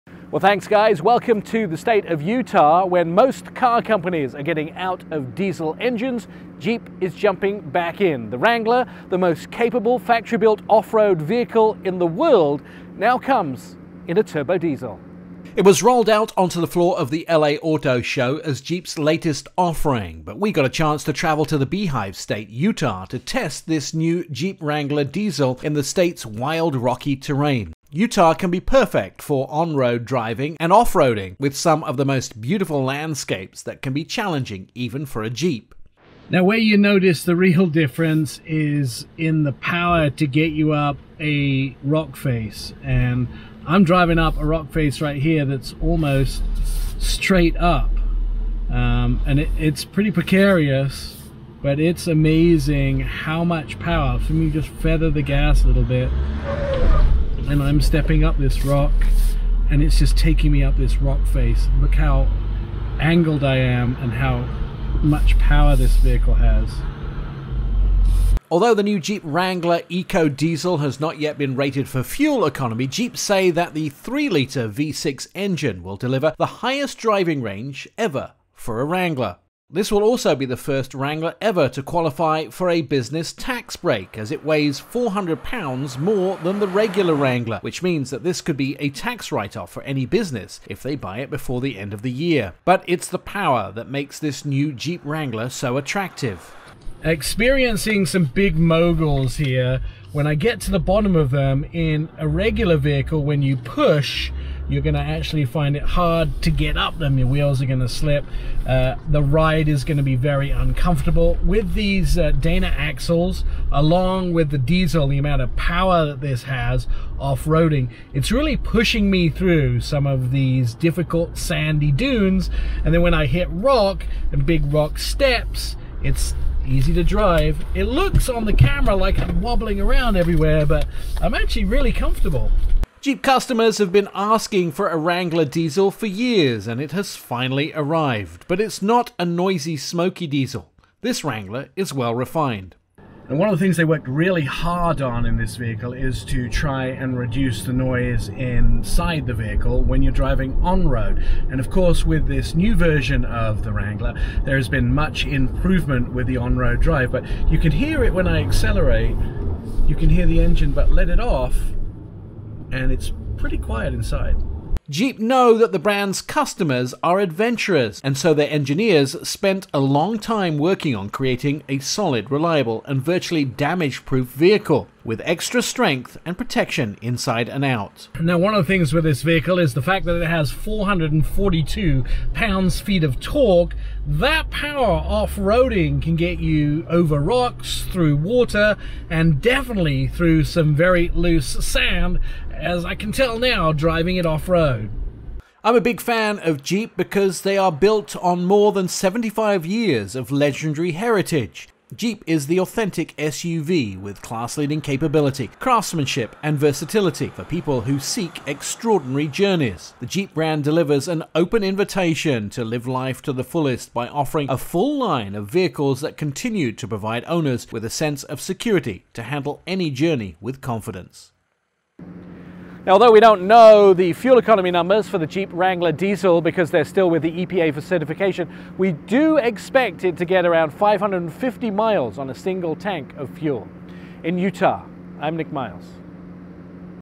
JEEP Wrangler Turbo Diesel
Nik Miles is in Utah test driving the all new 2020 Jeep Wrangler Turbo Diesel.